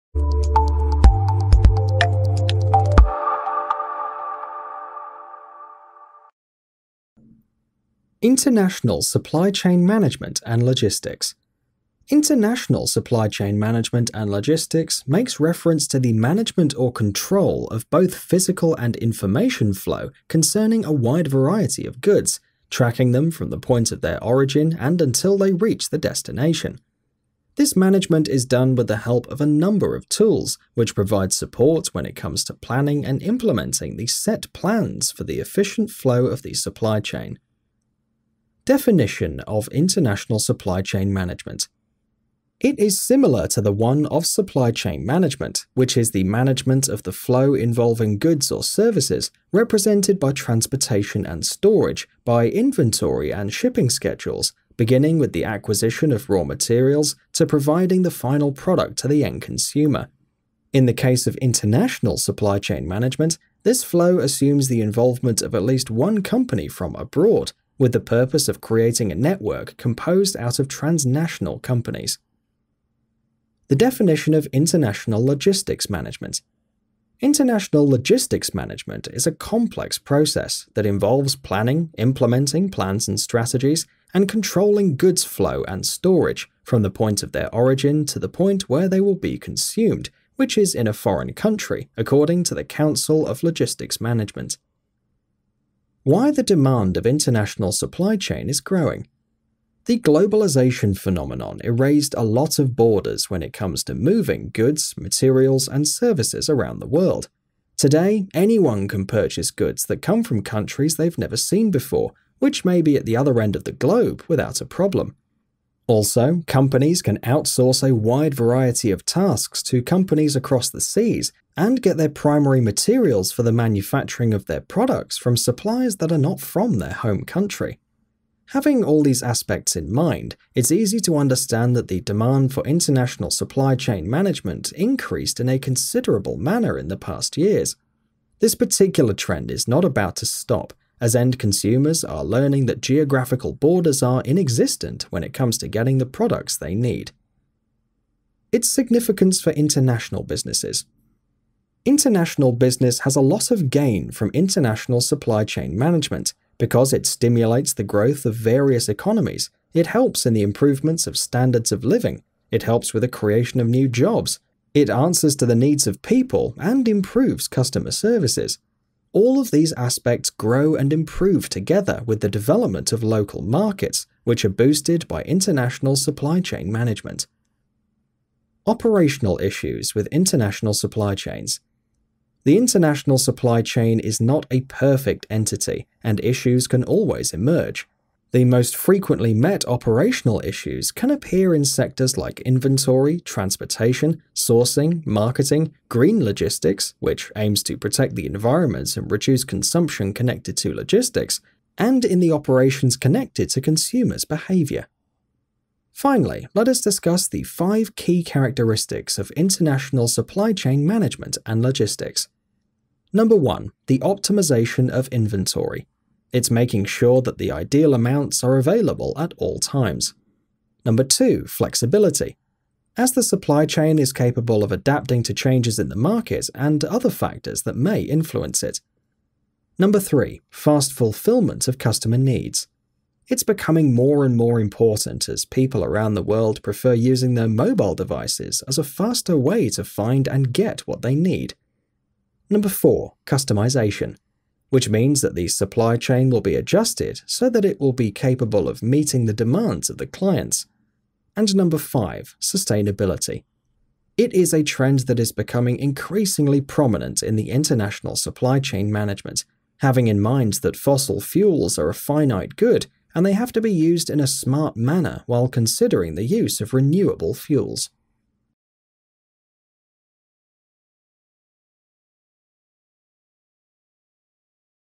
What is International Supply Chain Management? and How Does it Work? AIMS Education
International supply chain management refers to the coordination and management of the flow of goods, services, and information across international borders. It encompasses the planning and control of logistical activities, sourcing, procurement, production, and distribution processes. The objective of international supply chain management is to optimize efficiency, reduce costs, and ensure timely delivery of products to meet global demand. This involves navigating complexities such as varying regulations, tariffs, currency fluctuations, and cultural differences. Effective international supply chain management is crucial for companies looking to compete in the global marketplace and meet the expectations of a diverse customer base.

Free Lecture on International Supply Chain Management and International Logistics Management, and for more Lectures